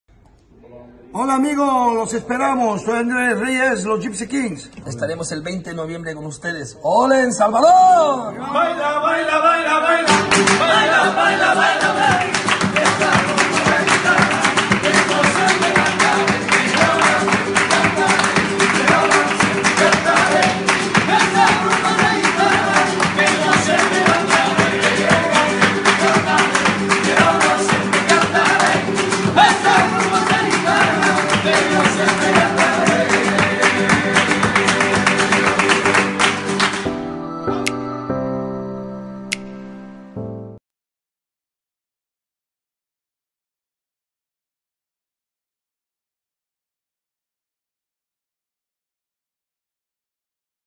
Gipsy Kings se presentarán en El Salvador
Los Gipsy Kings son una formación musical francesa gitana, cuyos miembros tienen ascendientes españoles​ y que canta en Idioma español una mezcla entre flamenco, pop y varias tradiciones de canción ligera como la rumba catalana.